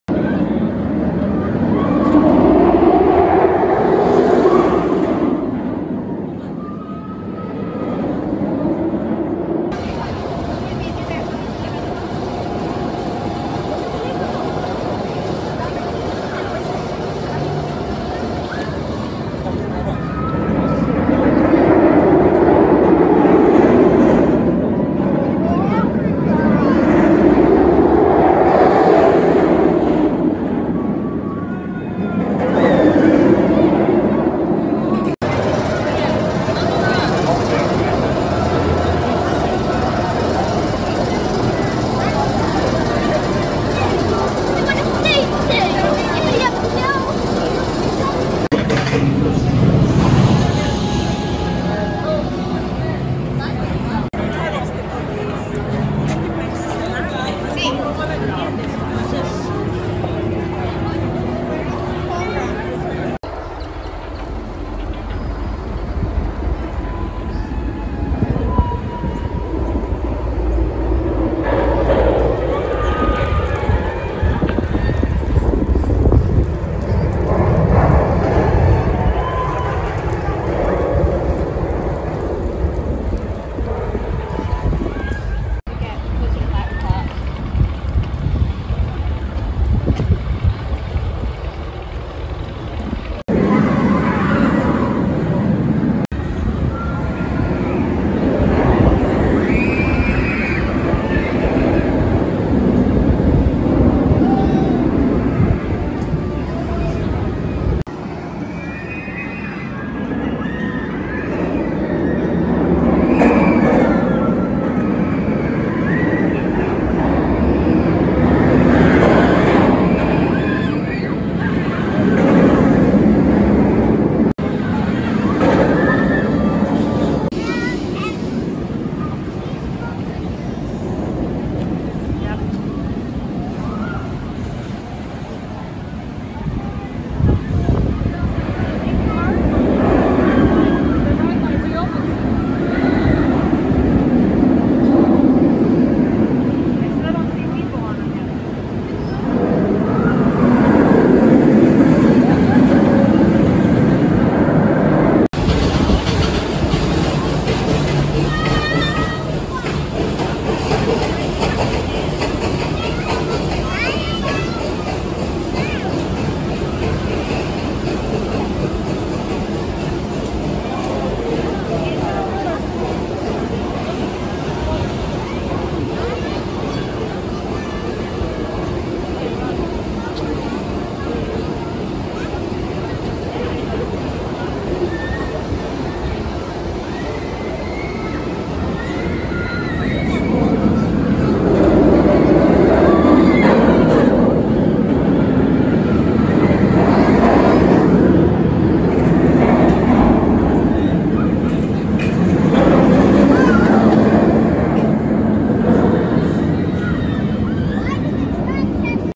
Montu @ Busch Gardens Tampa Bay | Off-Ride
Montu is the large B&M Inverted roller coaster at Busch Gardens Tampa Bay. Often called the best B&M Invert in North America, how good is this coaster - in my opinion? Since I seem to generally dislike B&M inverted coasters (I'm team SLC since Thailand has an SLC and not a B&M Invert), my opinion may not be the same as other enthusiasts. However, I generally find this ride enjoyable with a few stand out elements that make it an enjoyable ride -- I just don't enjoy it enough to rank it as a truly special coaster like most others will.

Montu เป็นรถไฟเหาะ B&M Inverted ขนาดใหญ่ที่ Busch Gardens Tampa Bay มักเรียกกันว่า B&M Invert ที่ดีที่สุดในอเมริกาเหนือ รถไฟเหาะนี้ดีแค่ไหน - ในความคิดของฉัน เนื่องจากโดยทั่วไปแล้วฉันดูเหมือนจะไม่ชอบ B&M กลับด้าน (ฉันเป็นทีม SLC เนื่องจากประเทศไทยมี SLC ไม่ใช่ B&M กลับด้าน) ความคิดเห็นของฉันอาจไม่เหมือนกับผู้ที่ชื่นชอบคนอื่นๆ อย่างไรก็ตาม โดยทั่วไปแล้วฉันพบว่าเครื่องเล่นนี้สนุกด้วยองค์ประกอบที่โดดเด่นบางประการที่ทำให้เครื่องเล่นนี้สนุก ฉันแค่ไม่สนุกพอที่จะจัดอันดับให้เป็นรถไฟเหาะที่พิเศษจริงๆ อย่างที่คนอื่นๆ ส่วนใหญ่ชอบ ค้นหาความคิดของฉันเกี่ยวกับรถไฟเหาะที่น่าประทับใจนี้ในรีวิวนี้!

0:00 - 0:32 Immelmann
0:10 - 0:35 First Drop and Immelmann
0:35 - 0:48 Chain Lift
0:48 - 1:03 Long Lines
1:03 - 1:33 First Part (Parking Lot View)
1:33 - 1:47 Turn By Queue/Off Brake Run
1:47 - 2:08 Loop and Immelmann
2:08 - 2:35 Brake Run and Turn By Queue
2:35 - 2:53 Chain Lift
2:53 - 3:28 Out of Focus Drop and Inversions